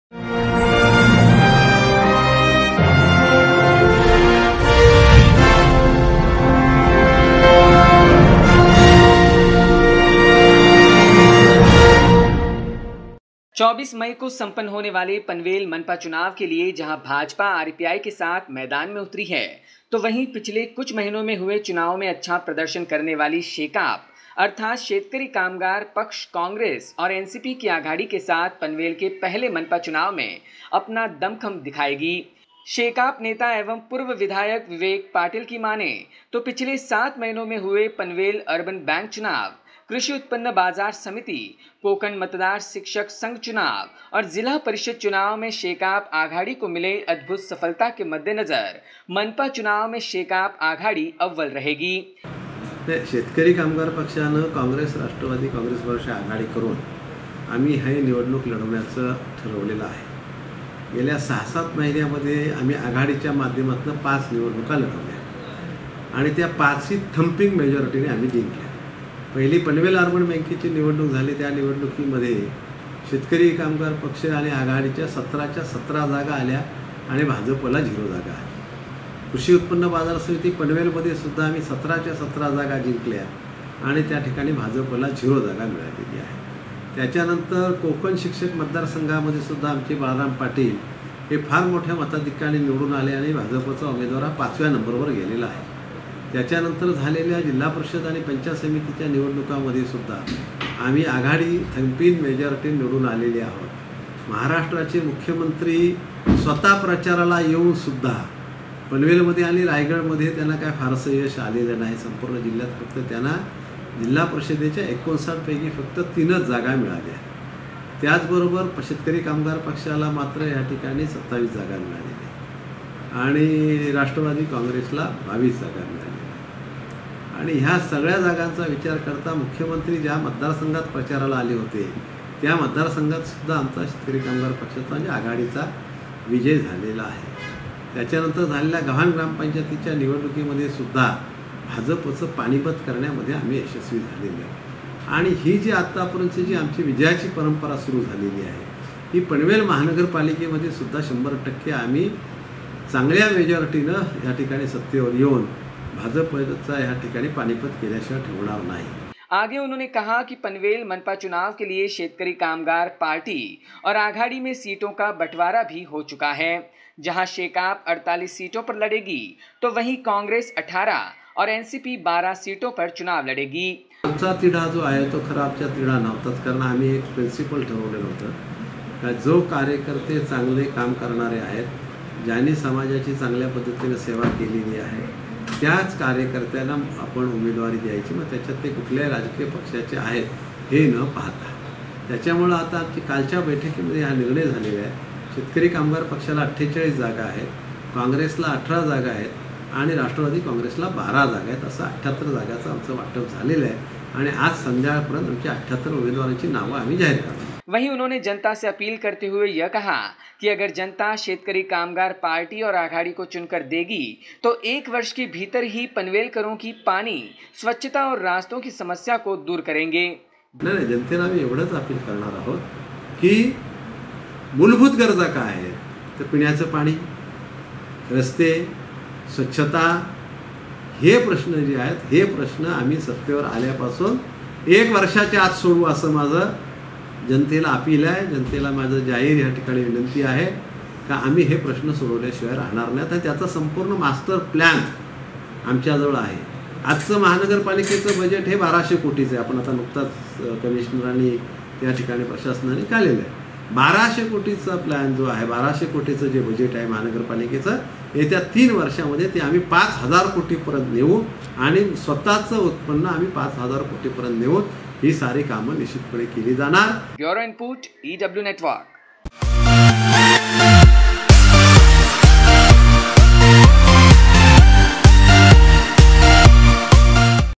PWP-CONG-NCP will win with a thumping majority in Panvel Municipal Elections says Ex.Mla Vivek Patil
PWP-CONG-NCP Alliance will win with a thumping majority in Panvel Municipal Elections says Ex.Mla Vivek Patil